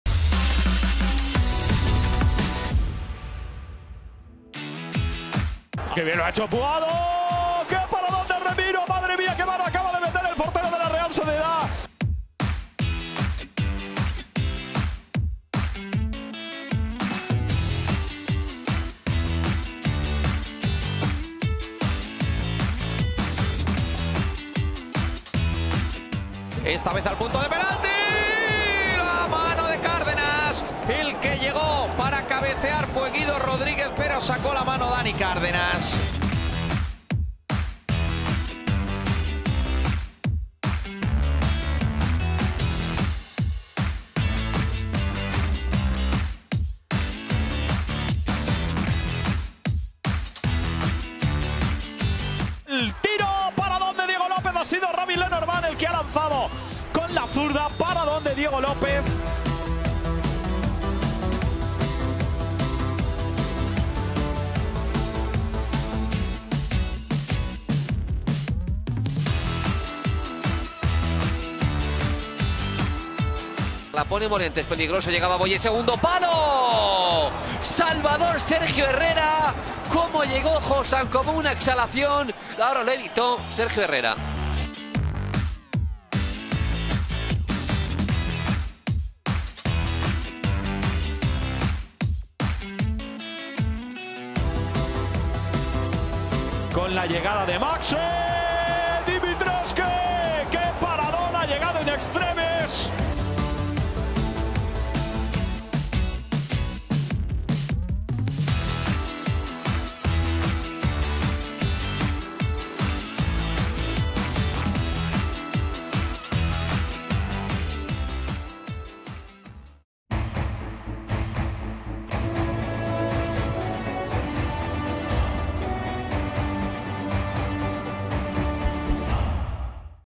TOP Paradas Jornada 15 LaLiga Santander 2021/2022
00:05 5.- Álex Remiro, Real Sociedad
00:26 4.- Dani Cárdenas, Levante UD
00:50 3.- Diego López, RCD Espanyol
01:14 2.- Sergio Herrera, CA Osasuna
01:35 1.- Stole Dimitrievski, Rayo Vallecano